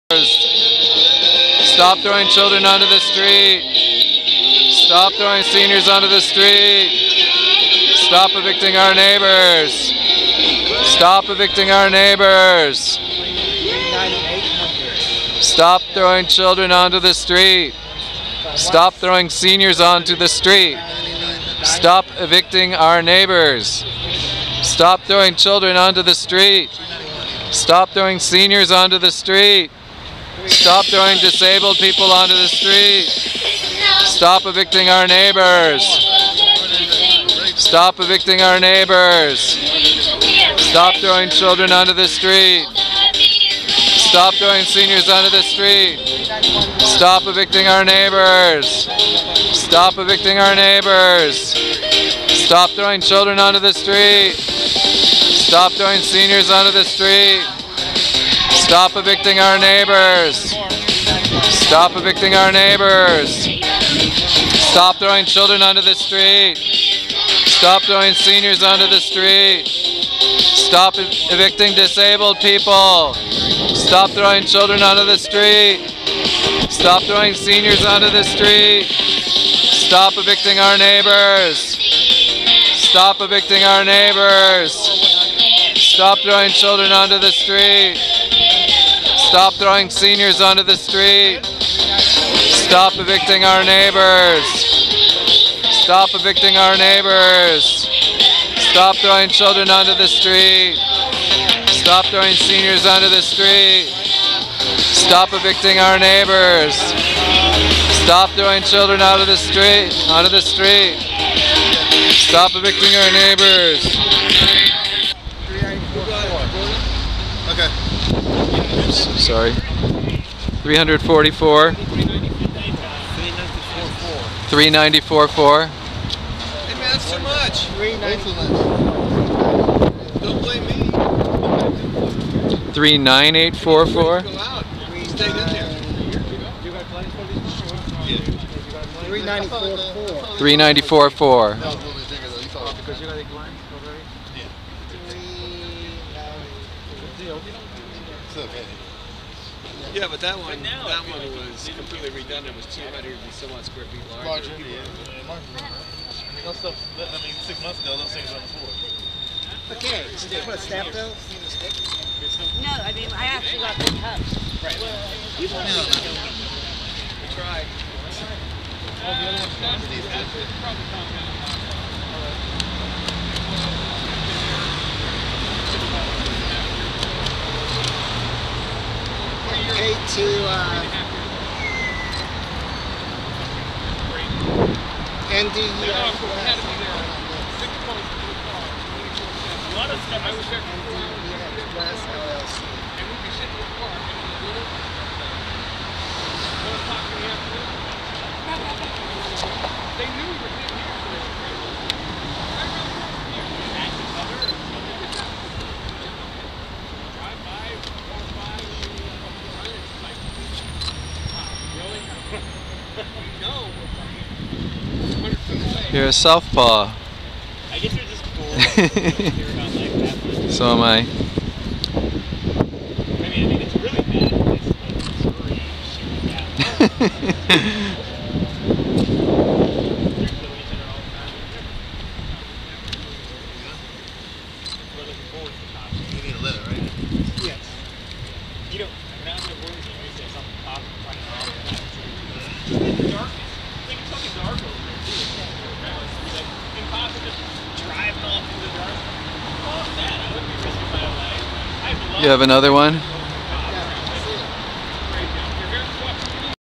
Part IV of Occupy the Auctions on February 6, 2013, in San Francisco
Part IV of Occupy the Auctions on February 6, 2013, in San Francisco (VID00004.MOV)